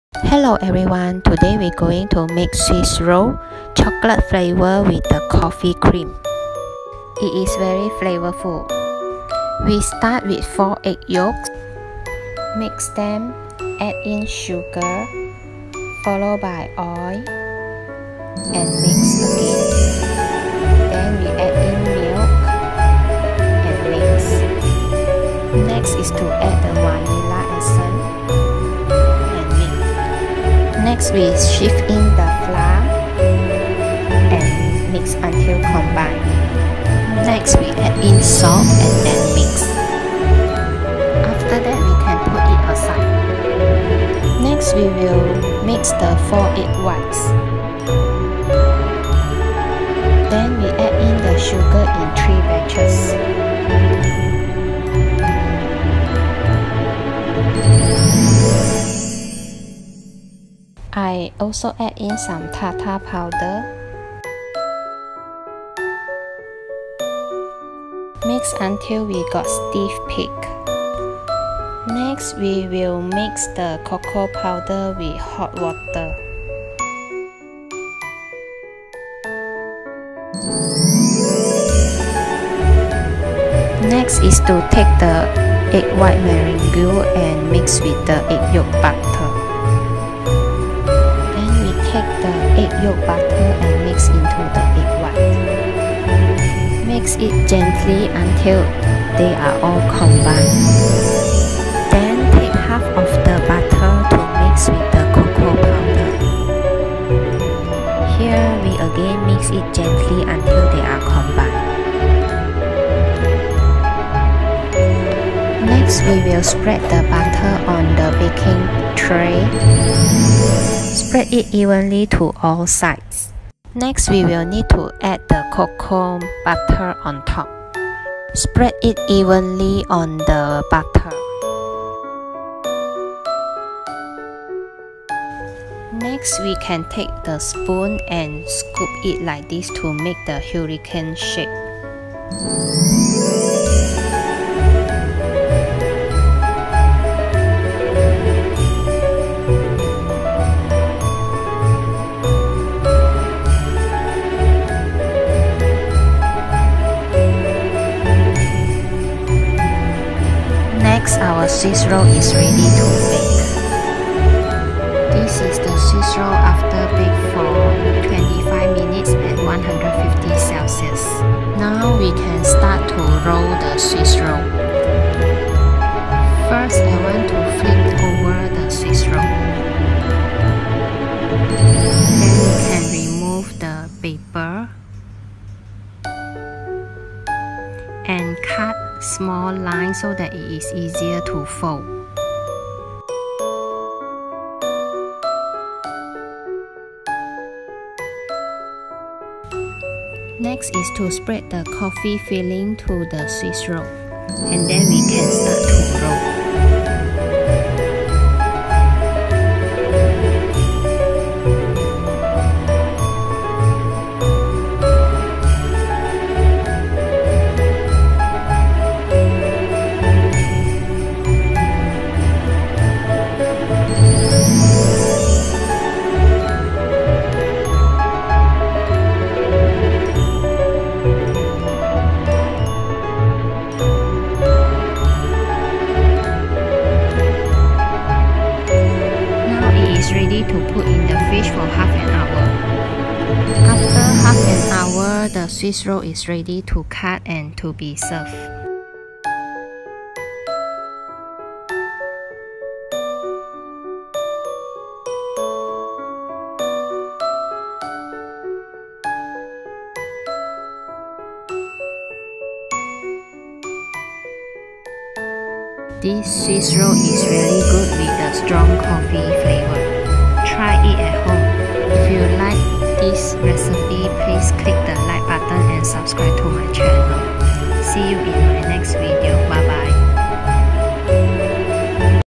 CHOCOLATE VANILA SWISS ROLL with COFFEE CREAM FILLINGS | Swiss roll recipe | Mini Swiss Roll
Ingredients:-
8inch square pan

(A)
4 egg yolks
1tbsp of caster sugar
1/4cup of vegetable oil
1/4 cup of milk
1/2 tsp of vanila essence
1/3 cup of cake flour ( can be replace with all purpose flour)

(B)
4 egg whites
3tbsp of caster sugar
1/2 tsp of Tartar powder

2 1/2 tsp of cocoa powder
1tbsp of warm water

(D)
2 1/2 tsp of Nescafé powder
1tbsp of hot water
5tbsp of condense milk

LIVE.LOVE.HUGS.YOU ♥